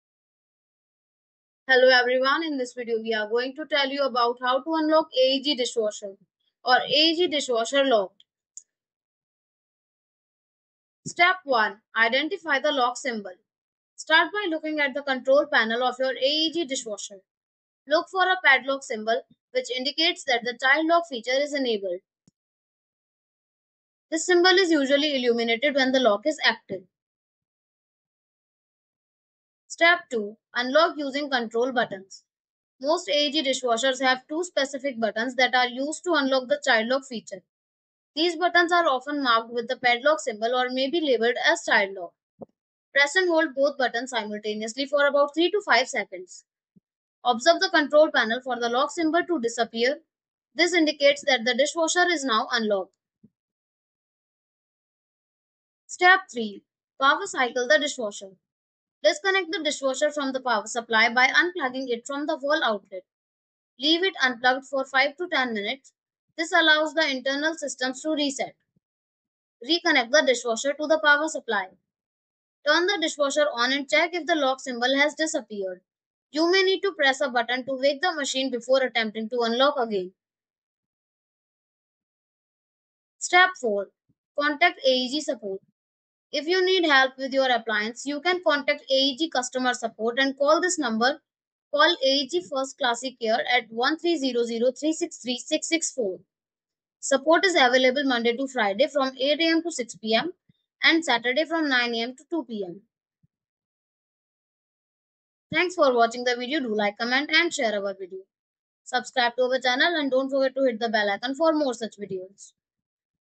How To Unlock AEG Dishwasher (AEG Dishwasher Locked)
Are You Struggling With A Locked Aeg Dishwasher? We Will Outline The Necessary Steps To Unlock The Dishwasher. Our Expert's Advice Will Help You Out To Troubleshoot Any Issues With Your Appliance.

00:11 - Step 1: Identify The Lock Symbol
00:29 - Step 2: Unlock Using Control Buttons
00:56 - Step 3: Power Cycle The Dishwasher
01:23 - Step 4: Contact AEG Support